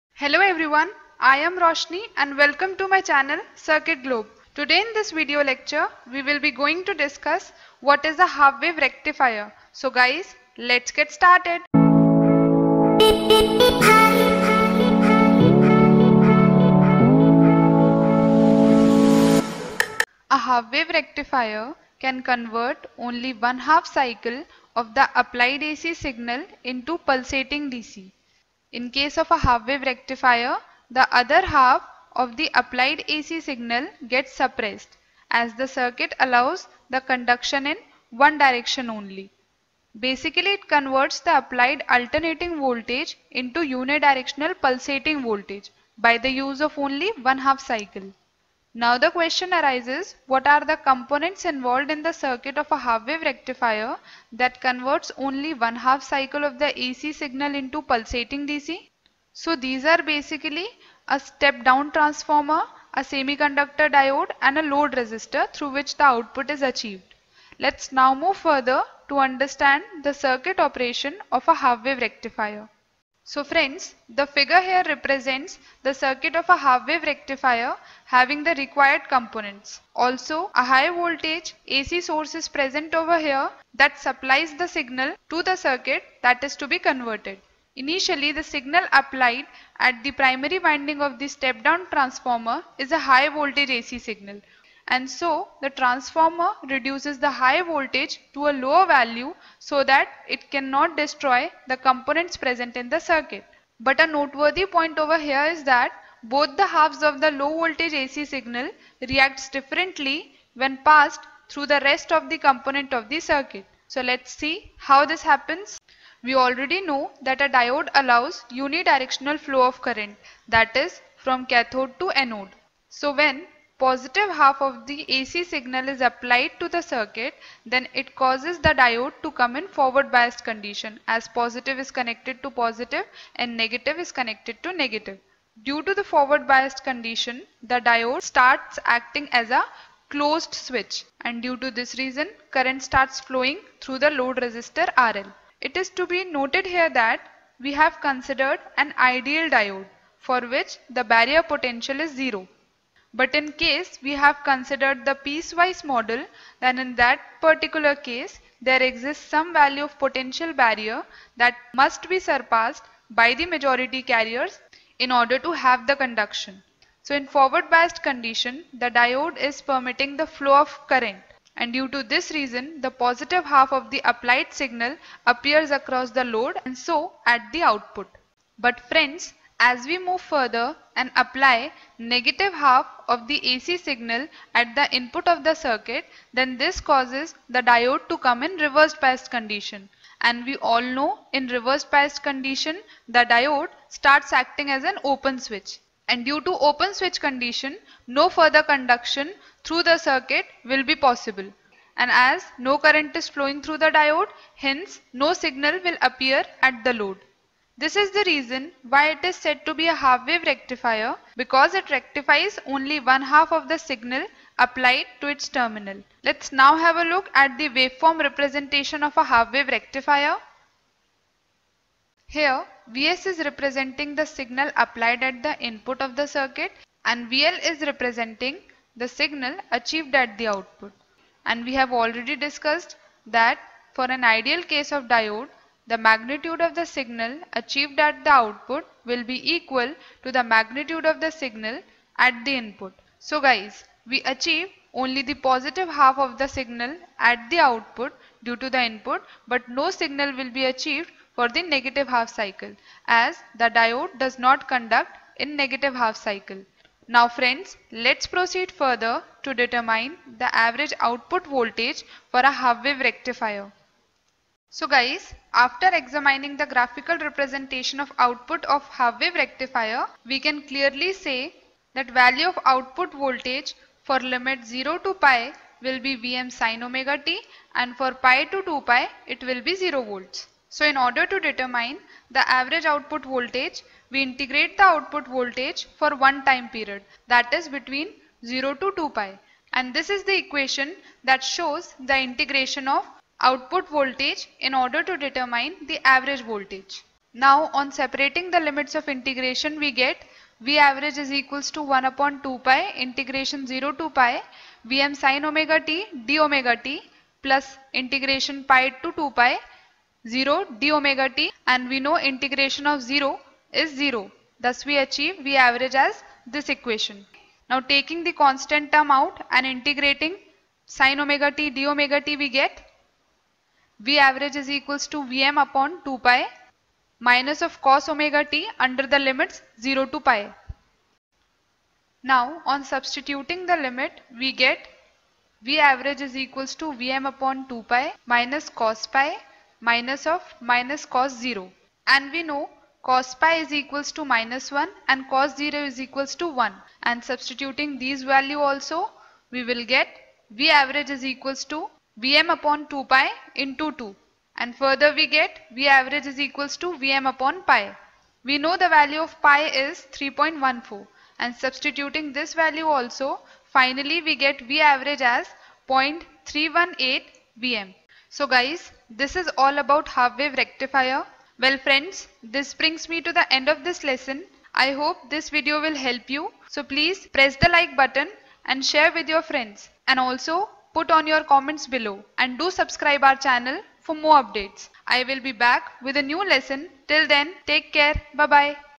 What is a Half Wave Rectifier? Circuit, Working and Waveform Representation
This video leacture is based on Half Wave Recifiers. In this video you will get to know about the circuit representation and working of half wave rectifier. Along with this you will also have the derivation of equation for average output voltage.